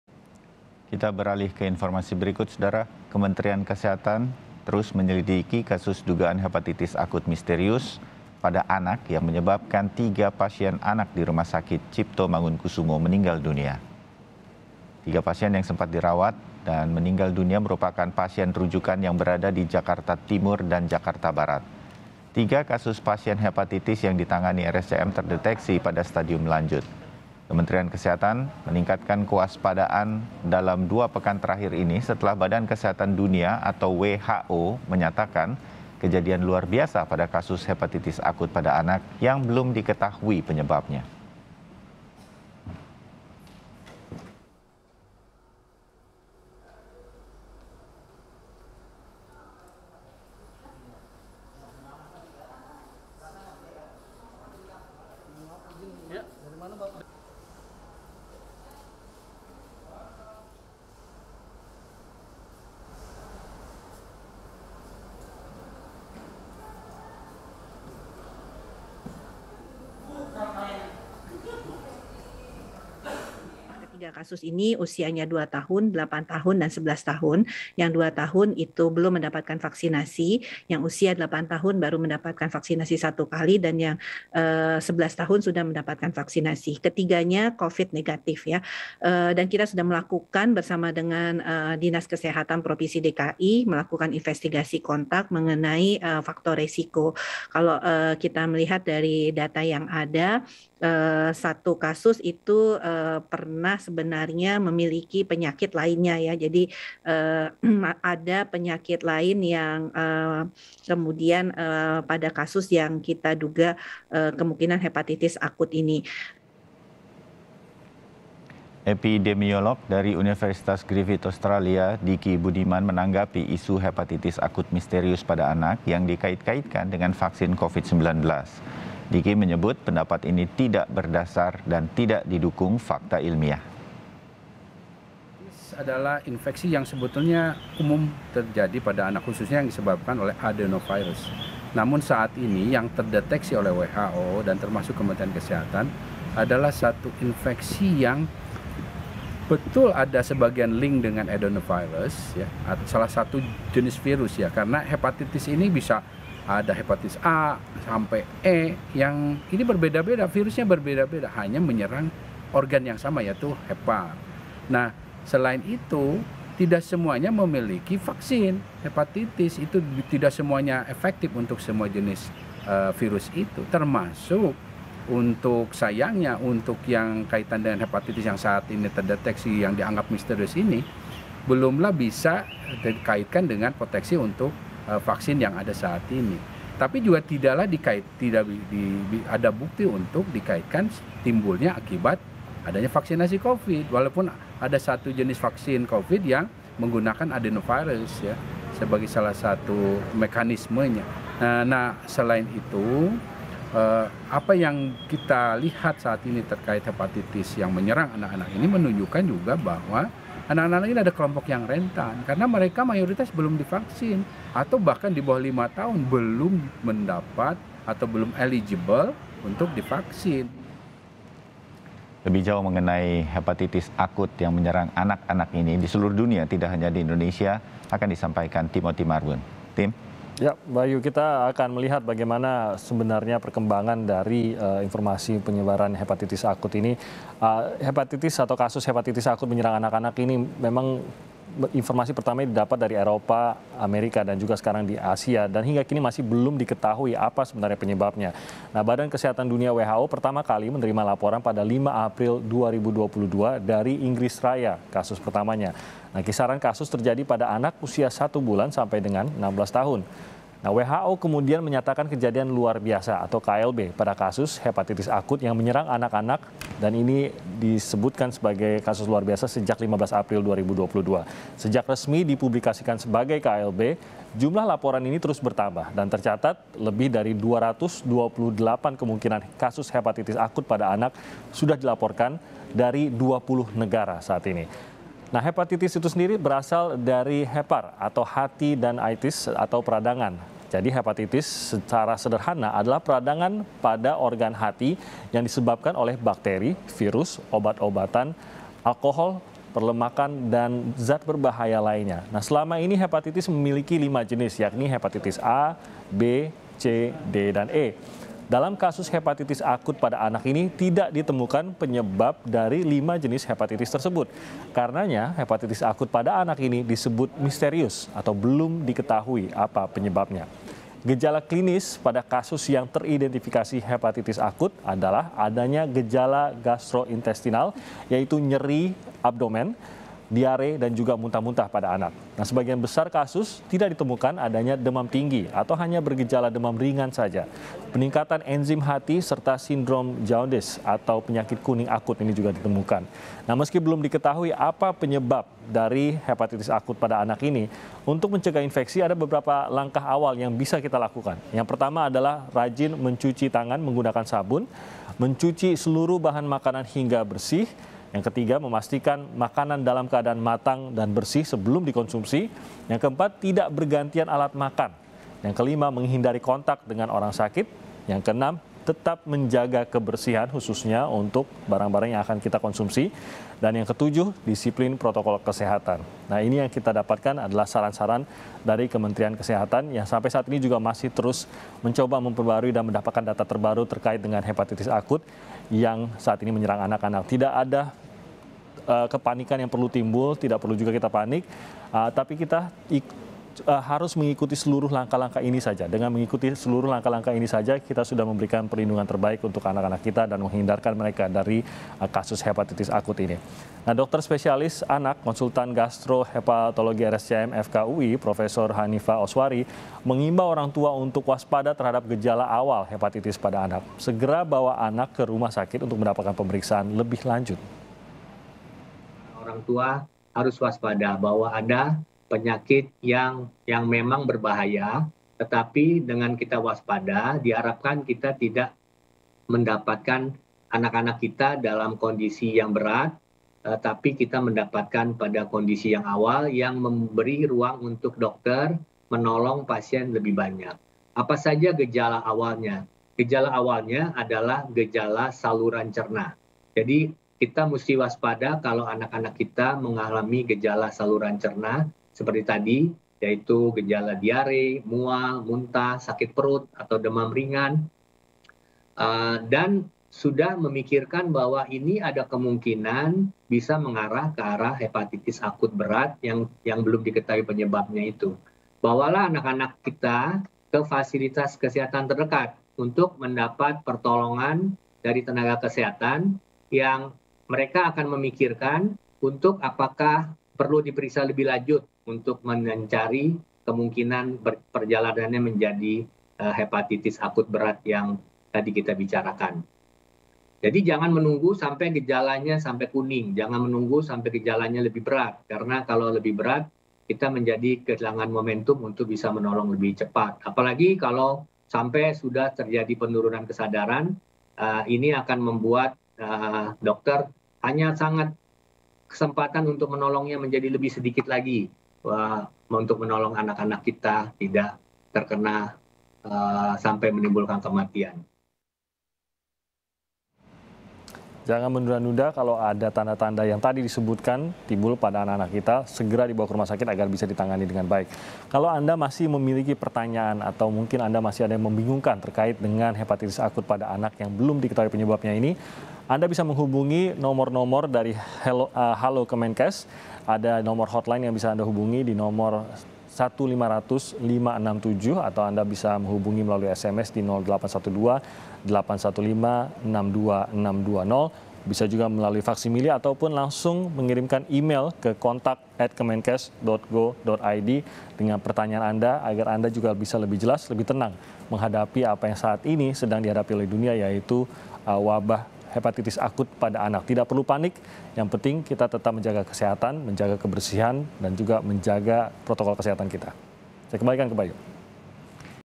Waspadai Hepatitis Akut pada Anak, Perhatikan Gejala Seperti Ini!
JAKARTA, KOMPAS.TV - Kementerian Kesehatan sedang menyelidiki kasus dugaan hepatitis akut misterius pada anak yang menyebabkan tiga pasien anak di Rumah Sakit Cipto Mangunkusumo meninggal dunia.

Ketiga pasien yang sempat dirawat dan meninggal dunia merupakan pasien rujukan yang berada di Jakarta Timur dan Jakarta Barat.

Tiga kasus pasien hepatitis yang ditangani RSCM terdeteksi pada stadium lanjut.

Kementerian Kesehatan meningkatkan kewaspadaan dalam dua pekan terakhir setelah badan kesehatan dunia atau WHO menyatakaan kejadian luar biasa pada kasus hepatitis akut pada anak yang belum dikatehui penyebabnya.

Epidemiolog dari Universitas Griffith, Dicky Budiman menanggapi isu hepatitis akut misterius pada anak yang dikait-kaitkan dengan vaksin covid-19.

Dicky menyebut, pendapat ini tidak berdasar dan tak didukung fakta ilmiah.